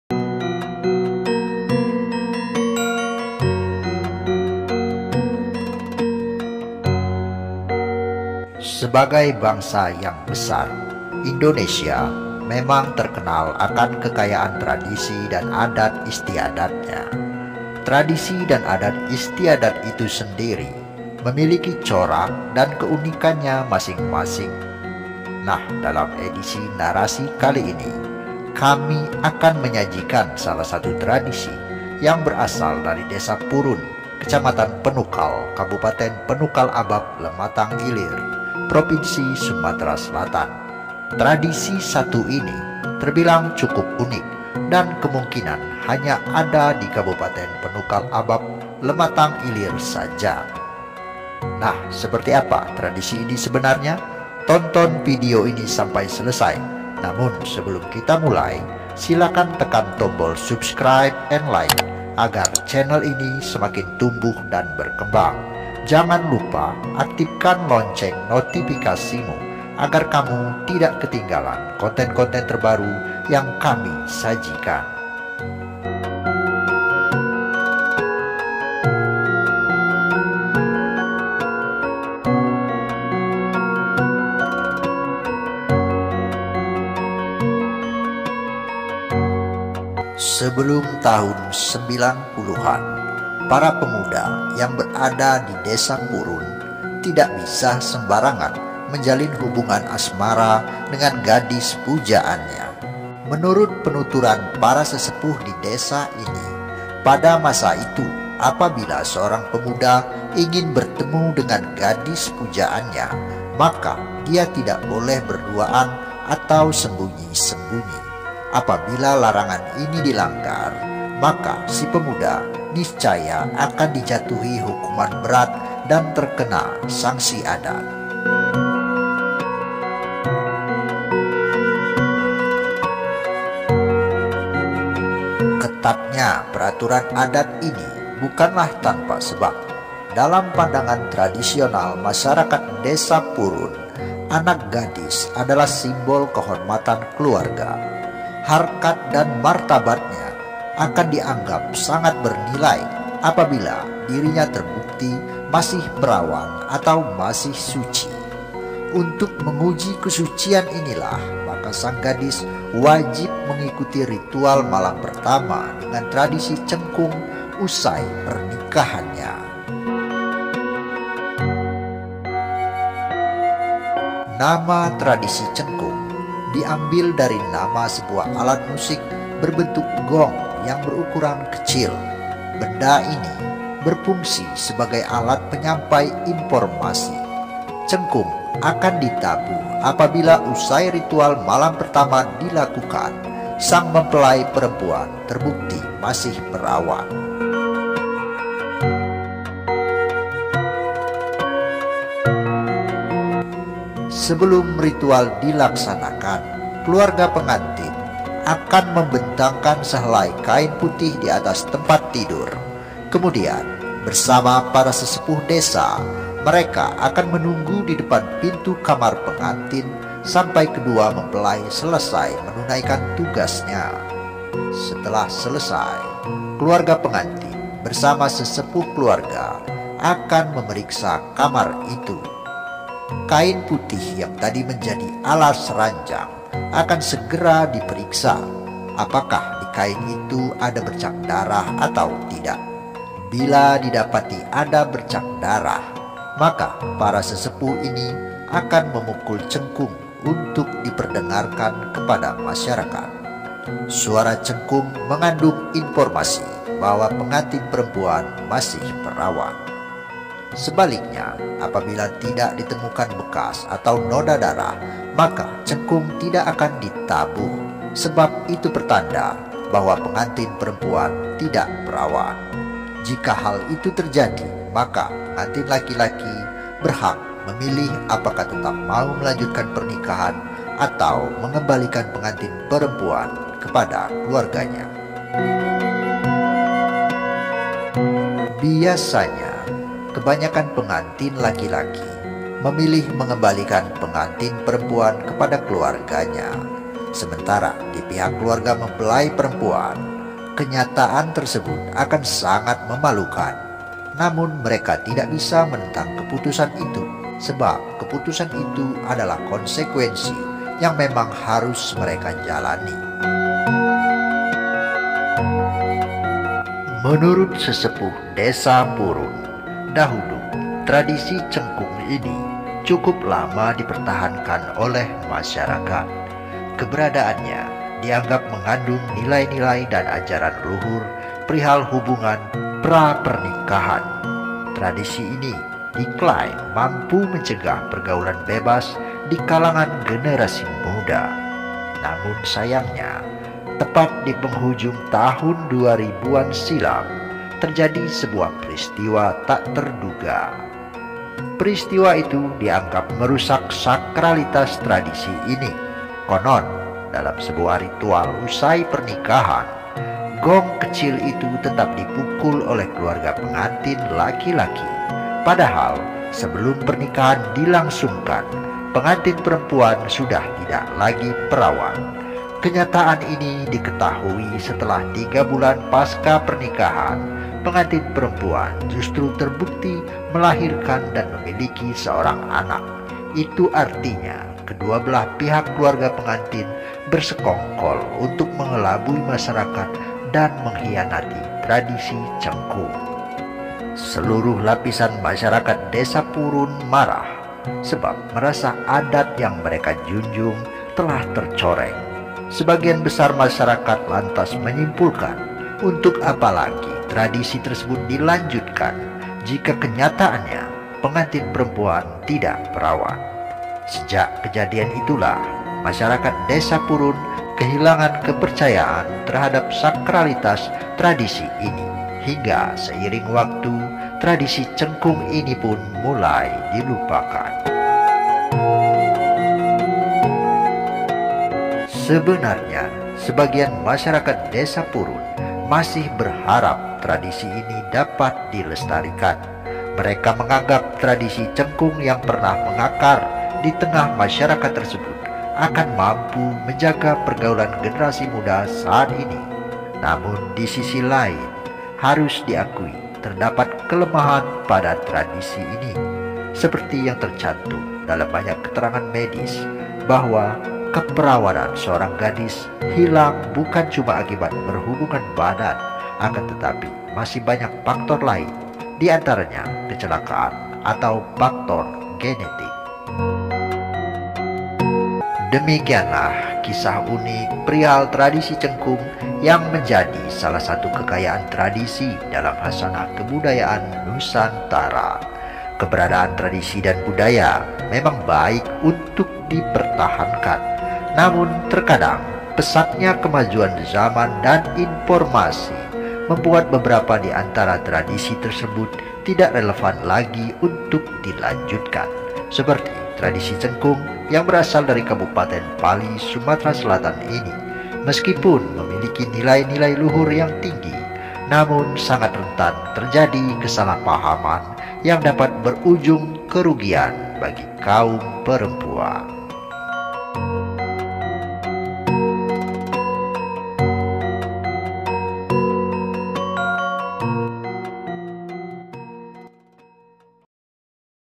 CENGKUNG: Ritual Pengantin Baru dan Darah Malam Pertama
TRADISI CENGKUNG: RITUAL PENGANTIN BARU DAN DARAH MALAM PERTAMA

Konon dalam sebuah ritual, gong kecil itu akan dipukul oleh keluarga pengantin laki-laki usai malam pertama pengantin baru selesai dilakukan. Gong akan dipukul jika pengantin perempuan maih perawan. Sebaliknya, gong tidak akan dipukul jika pengantin perempuan ternyata sudah tidak lagi perawan. Kalau hal terakhir terbukti, maka pengantin laki-laki diberi pilihan: melanjutkan pernikahan atau menceraikan pengantin perempuan.

Inilah adat dan tradisi unik dari kabupayen penukal Abab Lematang Ilir (PALI) yang mungkin tak banyak yang mengetahui.